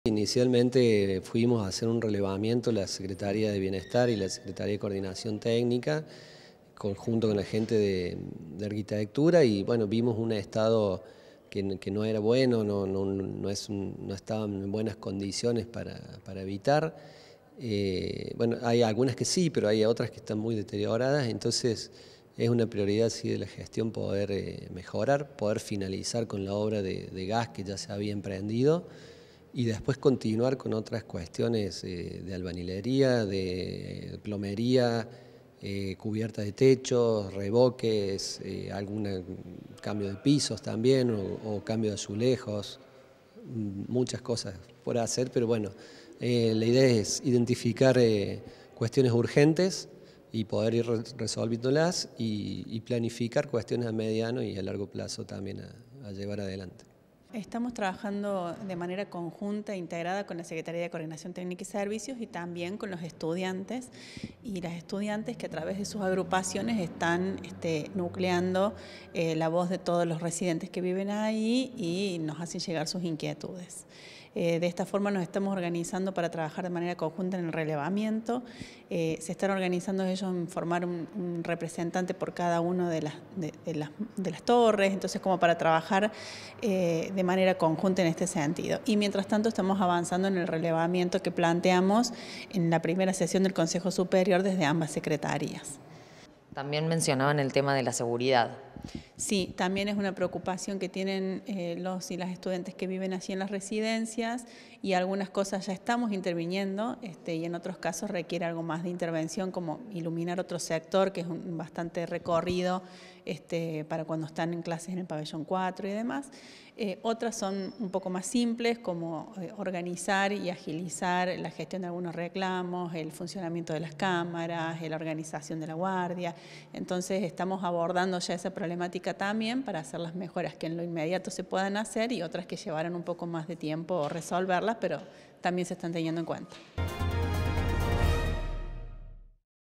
Residencias Universitarias: iniciarán arreglos tras diálogo con estudiantes
EZEQUIEL TARDIVO – SECRETARIO DE COORDINACIÓN TÉCNICA Y SERVICIOS UNRC

PAOLA BEASSONI – SECRETARIA DE BIENESTAR UNRC

TRAS EL DIÁLOGO CON ESTUDIANTES QUE LAS HABITAN, INICIARÁN CON URGENCIA ARREGLOS EN LAS RESIDENCIAS
UniRio TV
Universidad Nacional de Río Cuarto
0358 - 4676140